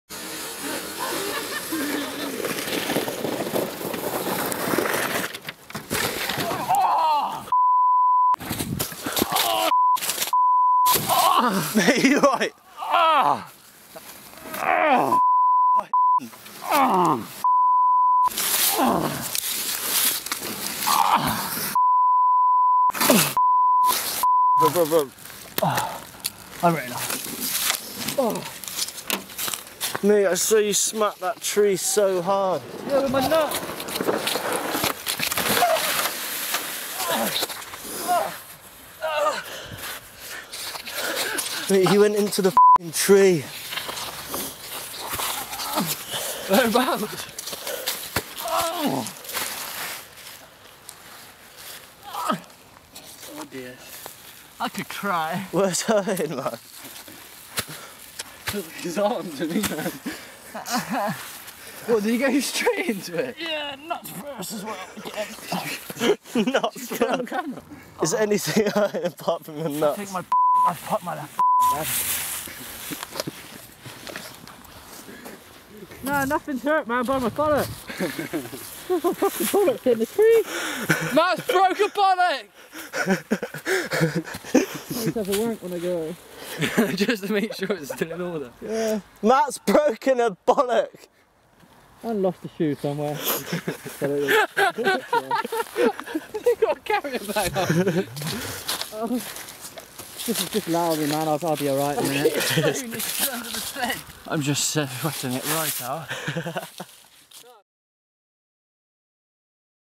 Matt Coles Crushed left Nut!
Some old footage found lurking in the hard drives.
This footage was recorded on the slopes of Henbury hill golf course in 2011.
Although Matt Cole never did find his left nut he luckily still has the other 2!. wait?